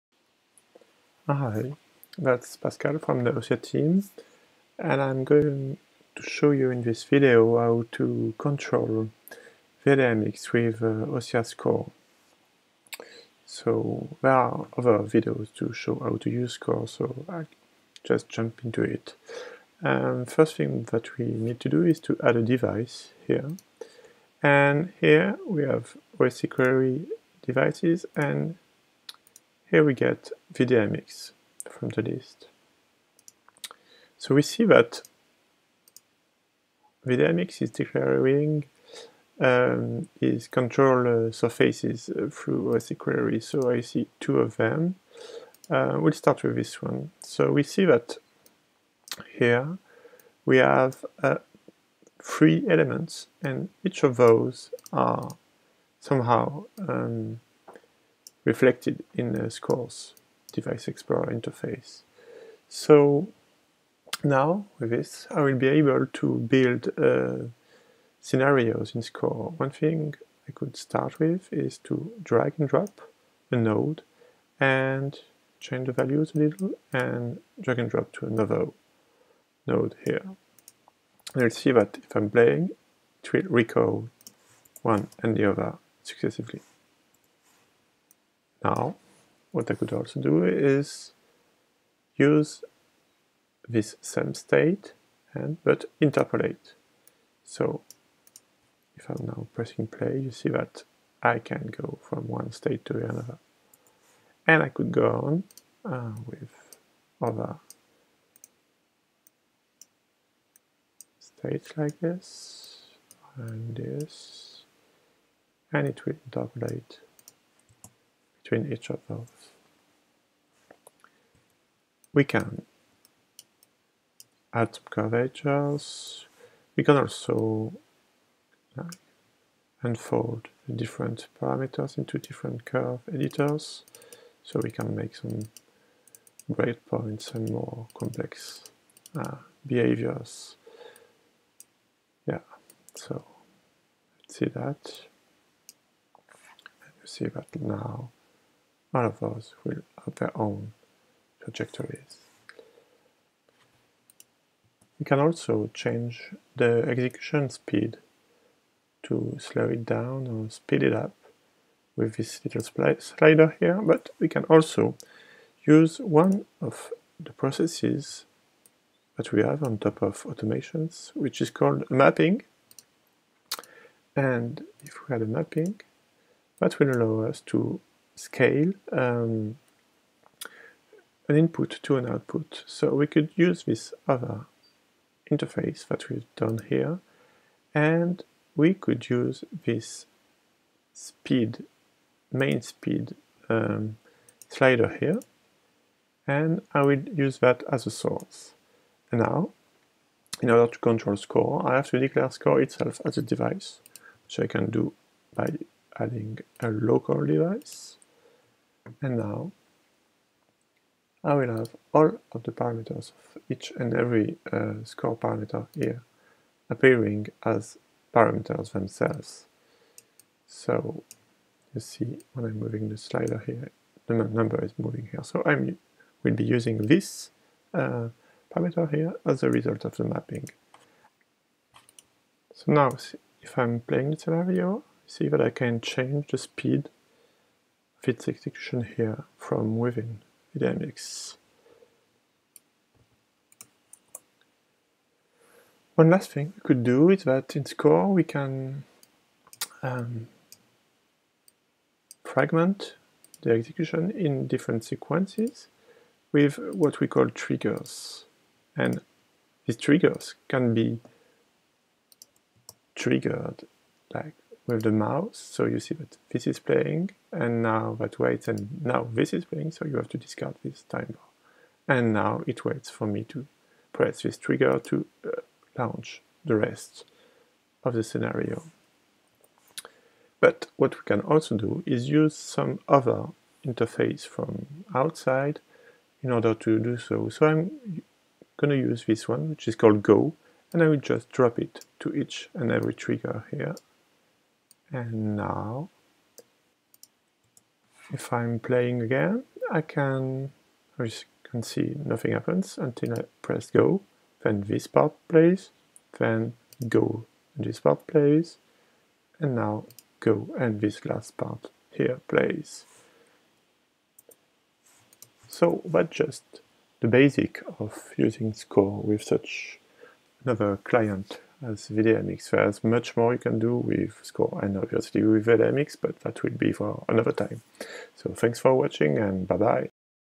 VDMX score demo
A short demo video showing how to use ossia score with VDMX's Controls Surfaces, via OSCQuery